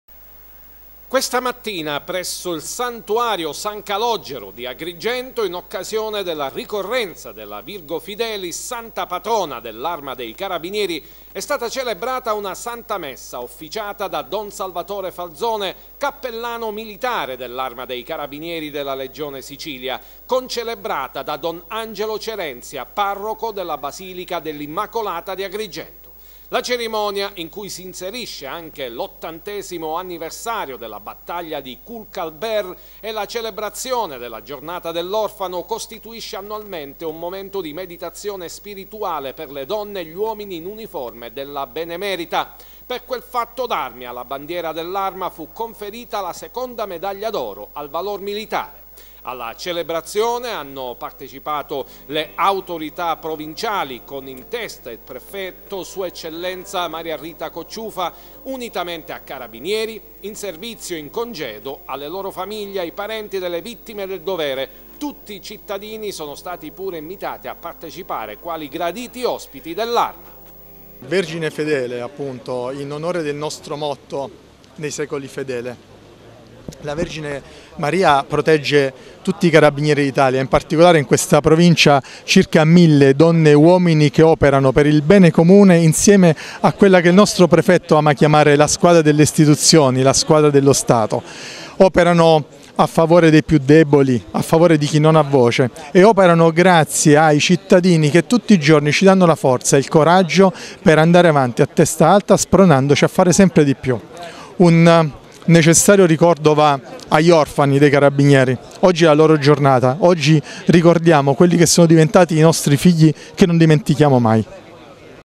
Agrigento, l'Arma dei Carabinieri celebra la "Virgo Fidelis" [STUDIO 98]
Nella chiesa di San Calogero ad Agrigento i Carabinieri celebrano la “Virgo Fidelis” la santa patrona dell’Arma dei Carabinieri.